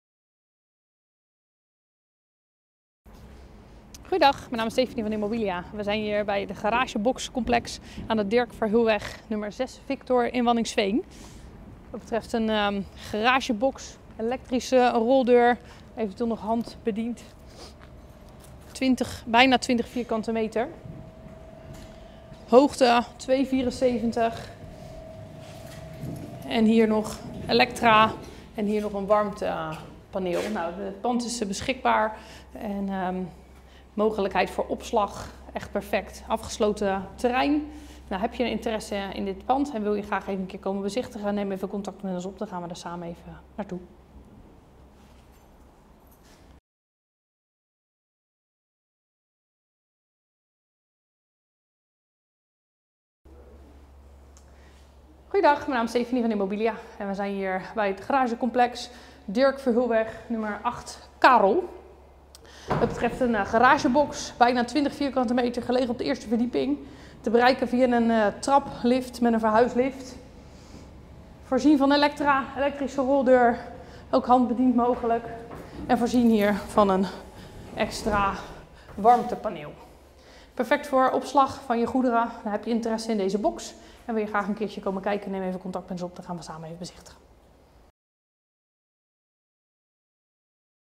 Verhuurd - Twee garageboxen gelegen op een keurig complex in Waddinxveen - immobilia.nl
Dirk Verheulweg 4 - 6V en 8K - Waddinxveen
Immobilia presenteert twee nieuwbouw opslagunits gelegen in Waddinxveen. Beide units maken onderdeel uit van project BPA12. BPA12 ligt op een toplocatie in Waddinxveen, op slechts 1 minuut rijden van de A12. Afslag Waddinxveen ligt centraal tussen Gouda en Zoetermeer. Het centrum van Waddinxveen is binnen enkele minuten te bereiken.
De opslagboxen zijn 24/7 beveiligd door middel van een camerasysteem. Gelegen op een afgesloten terrein, welke altijd toegankelijk is middels een automatische schuifpoort.

Unit 6V:
Ca. 20m2 en gelegen op de begane grond en is vanaf buiten te bereiken middels een elektrische overheaddeur. De unit is tevens voorzien van een infrarood paneel. De afmetingen van de box zijn circa: 2,24 x 3,09 x 5,75 (H x B x D)

Unit 8K:
Ca. 20m2 en gelegen op de eerste verdieping en beschikt over een schuine kap. Bereikbaar middels een trap en een goederenlift. De unit is tevens voorzien van een infrarood paneel. De afmeting bedragen 1,96 / 3,66 x 3,39 x 5,79 (H x B x D)

Huurtermijn:
In overleg met verhuurder.

Huuringangsdatum:
Per direct beschikbaar.

Huurprijsherziening:
Jaarlijks op basis van de CPI-index van het CBS.

Huurbetaling:
Per maand vooruit.

Waarborgsom:
Drie (3) maanden huur incl. BTW.

Huurovereenkomst:
Conform model ROZ.

BTW:
Verhuurder wenst te opteren voor BTW-belaste huur en verhuur. Ingeval huurder de BTW niet kan verrekenen zal de huurprijs worden verhoogd ter compensatie van de gevolgen van het vervallen van de mogelijkheid om te opteren voor BTW-belaste huur.